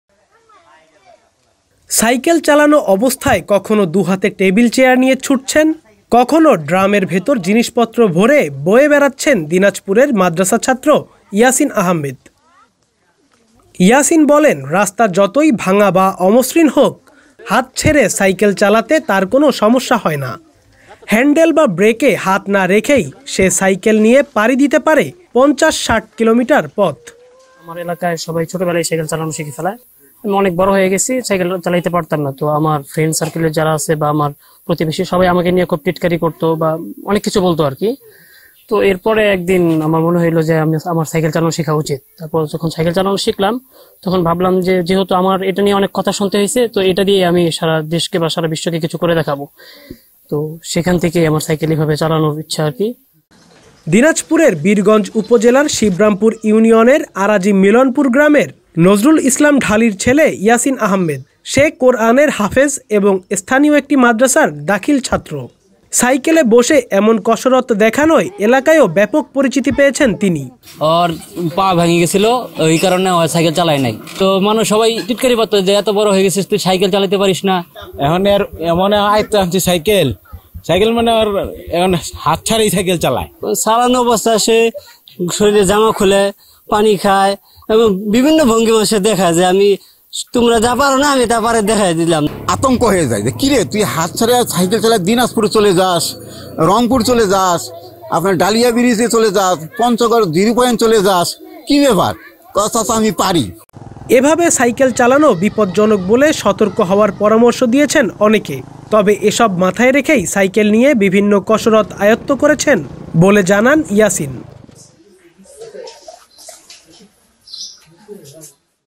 ইয়াসিনের সাইকেলে কসরত | bdnews24.com
দিনাজপুরে মাদ্রাসা ছাত্র ইয়াসিন সাইকেলে কসরত দেখিয়ে আলোড়ন তুলেছেন।